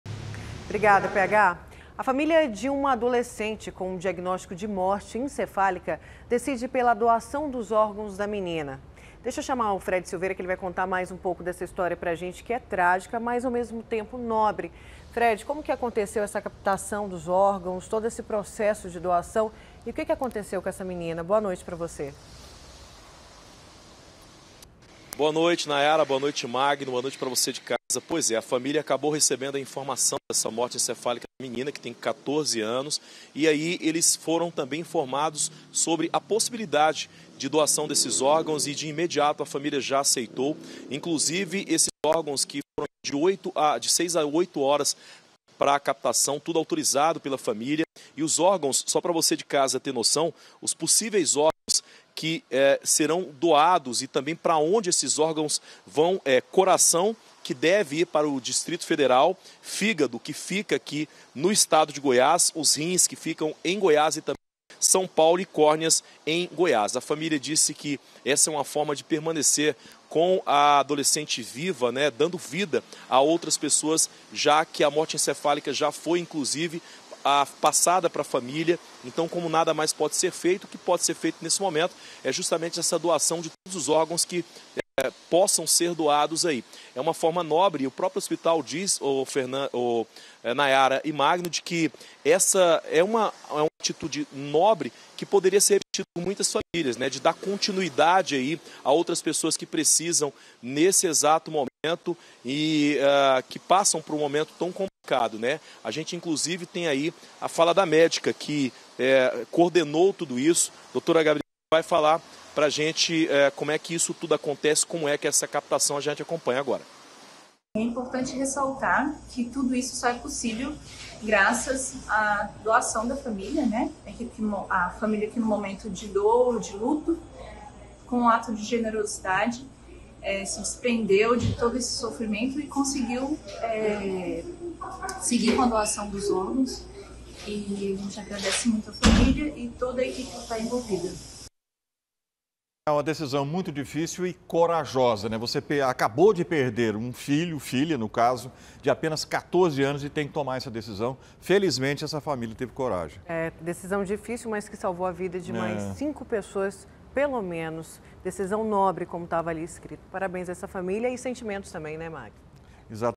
ATO NOBRE: FAMÍLIA DOA ÓRGÃOS DE ADOLESCENTE
A FAMÍLIA DE UMA ADOLESCENTE, COM DIAGNÓSTICO DE MORTE ENCEFÁLICA, DECIDE PELA DOAÇÃO DOS ÓRGÃOS DA MENINA.